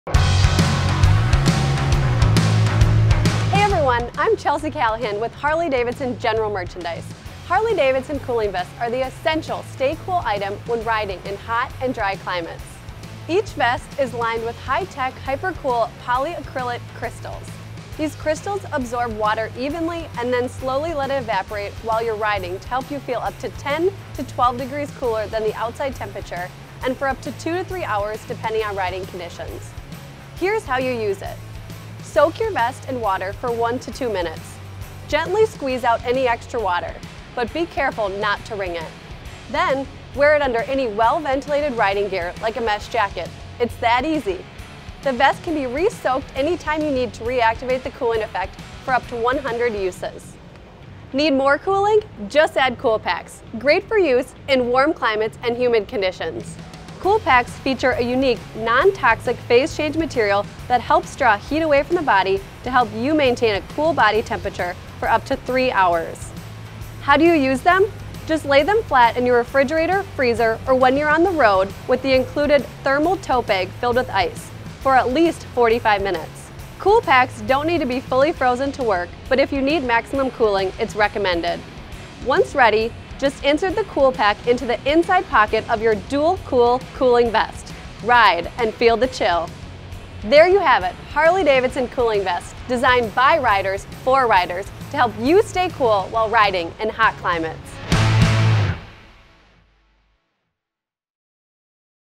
Dual Cool Cooling Vest & Cooling Kit
Chill out. The Dual Cool Cooling Vest & Cooling Kit doubles the chill for comfort in extreme heat. Designed with stretch panels for a close fit, the lightweight hydration vest features an embedded HyperKewl™ lining that absorbs water when you give it a quick soak (1-2 minutes). It slowly evaporates for a cooling effect that lasts 1-2 hours. For a greater cooling factor, lay inserts flat in the fridge, freezer, or cooler with ice water for a minimum of 45 minutes, then slip them in the interior pockets of the soaked vest. Expect 2-3 hours of refreshing coolness. A handy insulated tote stores your vest and four cool packs, plus room for a water bottle or two.

Stay Cool: Offers two ways to stay cool by soaking vest and using cool packs. Lining absorbs and holds water that slowly evaporates to provide a cooling effect for 2 hours. Vest is fully hydrated when soaked for 1-2 minutes. Can be rehydrated up to 100 uses. For even greater cooling, insert four non-toxic carbon based phase change cool packs into the four interior vest pockets. To activate cool packs, lay flat in the fridge, freezer, or cooler with ice water for a minimum of 45 minutes, then insert into the hydrated vest. Cool packs can be reused for up to 1000 uses.

Fit & Mobility: Contrasting stretch mesh panels provide a close fit.

Design Details: Rubber patch. Includes vest, four freezable cool packs, and thermal tote bag.

Materials: 100% nylon body with embedded HyperKewl™ lining.